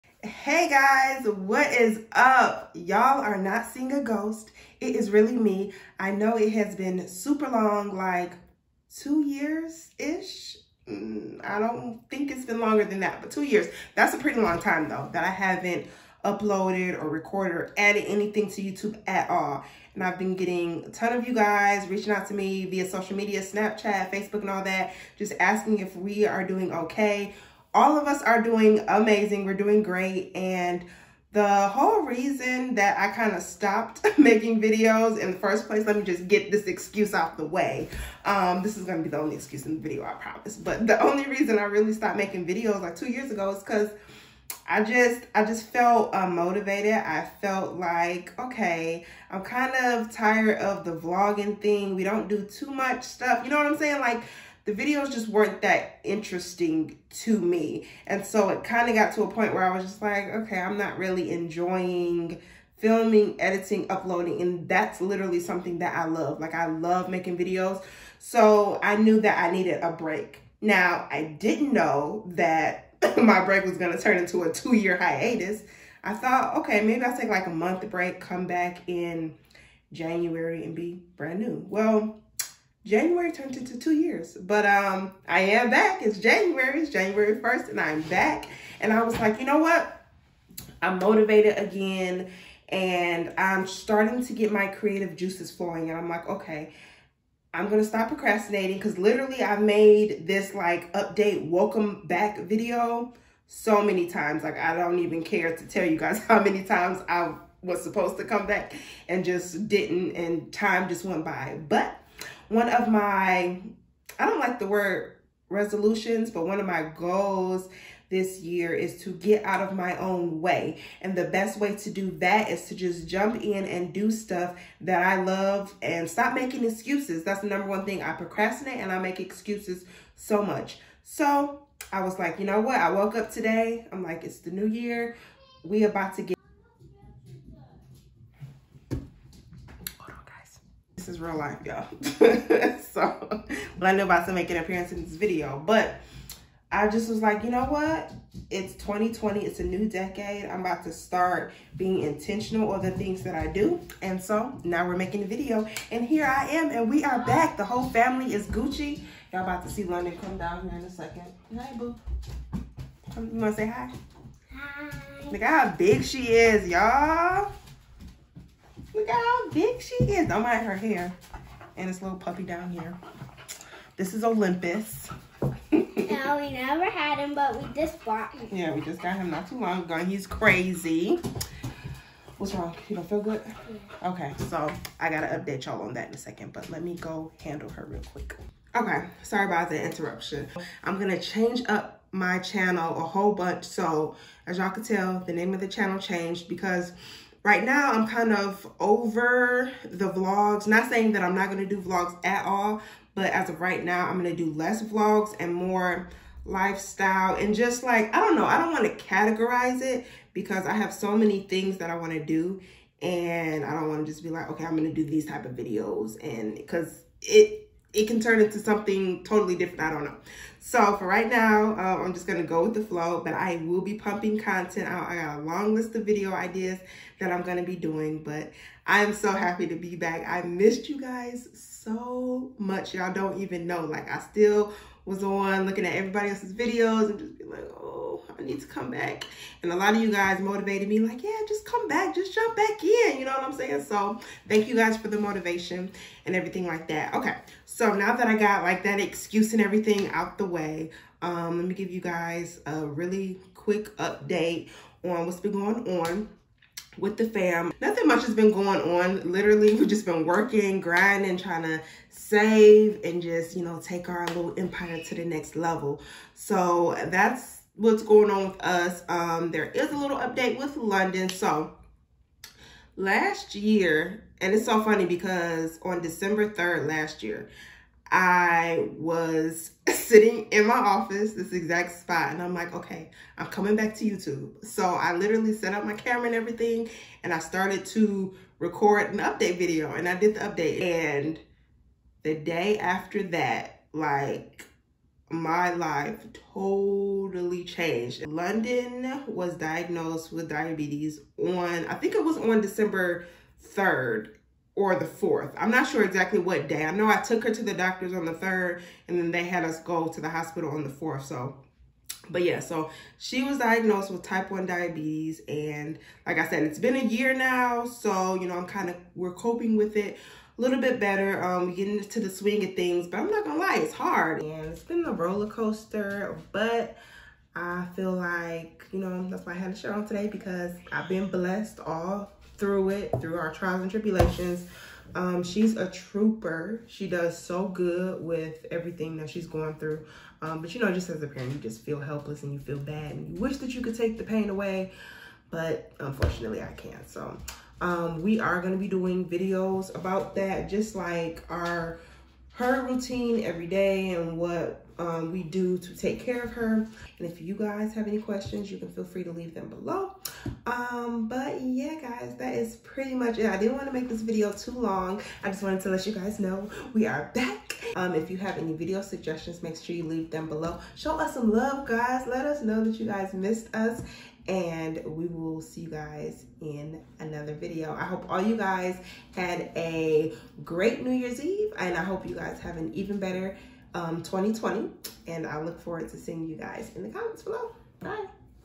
We've Been Gone Way Too Long!
Hey Guys! We are so happy to be back after 2 long years. Just a quick video to update you on where we have been. Show us some love!!!